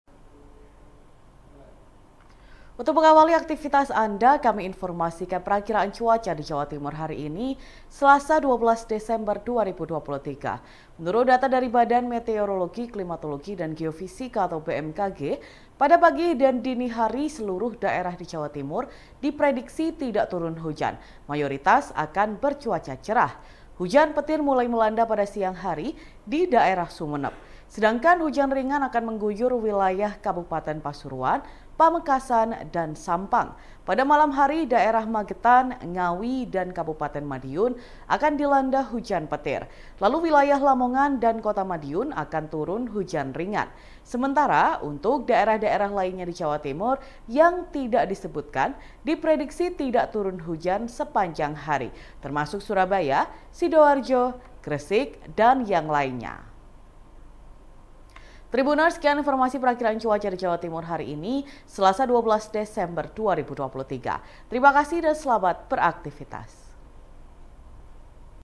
INFO CUACA SELASA 12 DESEMBER 2023
VP : sugix

Untuk mengawali aktivitas Anda, kami informasikan prakiraan cuaca di Jawa Timur, hari ini, Selasa 12 Desember 2023.

Menurut data dari Badan Meteorologi, Klimatologi, dan Geofisika atau BMKG, pada pagi dan dini hari seluruh daerah di Jawa Timur diprediksi tidak turun hujan, mayoritas akan bercuaca cerah.

Hujan petir mulai melanda pada siang hari di daerah Sumenep.

Sedangkan, hujan ringan akan mengguyur wilayah Kabupaten Pasuruan, Pamekasan, dan Sampang.

Pada malam hari, daerah Magetan, Ngawi, dan Kabupaten Madiun akan dilanda hujan petir.

Lalu, wilayah Lamongan dan Kota Madiun akan turun hujan ringan.

Sementara, untuk daerah-daerah lainnya di Jawa Timur yang tidak disebutkan, diprediksi tidak turun hujan sepanjang hari, termasuk Surabaya, Sidoarjo, Gresik dan yang lainnya.

Tribunners, sekian informasi prakiraan cuaca di Jawa Timur, hari ini, Selasa 12 Desember 2023.

Terima kasih dan selamat beraktivitas.